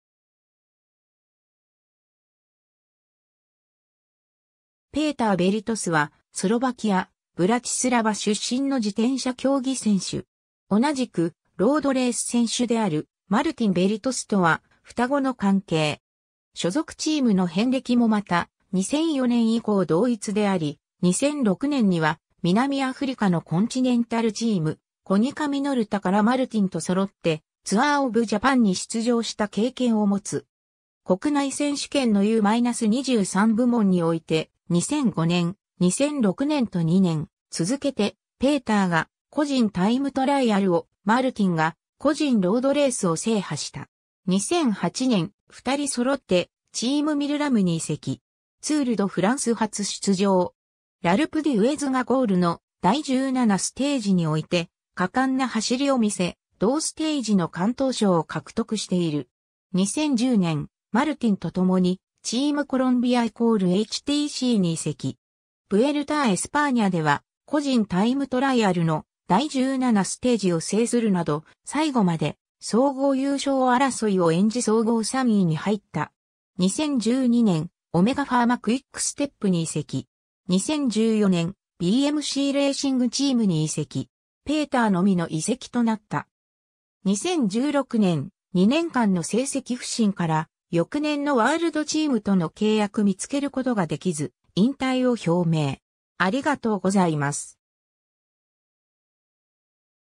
ペーター・ヴェリトス
ペーター・ヴェリトス（Peter Velits、1985年2月21日 - ）は、スロバキア、ブラチスラヴァ出身の自転車競技（ロードレース）選手。
 同じくロードレース選手である、マルティン・ヴェリトスとは双子の関係。
所属チームの遍歴もまた、2004年以降同一であり、2006年には南アフリカのコンチネンタルチーム「コニカミノルタ」からマルティンと揃ってツアー・オブ・ジャパンに出場した経験を持つ。
国内選手権のU-23部門において、2005年、2006年と2年続けて、ペーターが個人タイムトライアルを、マルティンが個人ロードレースを制覇した。
 2008年、二人揃ってチーム・ミルラムに移籍。
ツール・ド・フランス初出場。
ラルプ・デュエズがゴールの第17ステージにおいて果敢な走りを見せ、同ステージの敢闘賞を獲得している。
 2010年、マルティンとともに、チーム・コロンビア＝HTC（後の HTC - ハイ・ロード）に移籍。
ブエルタ・ア・エスパーニャでは、個人タイムトライアルの第17ステージを制する（この他、第1ステージのチームタイムトライアルでも区間優勝に貢献）など、最後まで総合優勝争いを演じ総合3位に入った。
 2012年、オメガファーマ・クイックステップに移籍。
 2014年、BMC・レーシングチームに移籍。
ペーターのみの移籍となった。
 2016年、2年間の成績不振から、翌年のワールドチームとの契約見つけることが出来ず、引退を表明。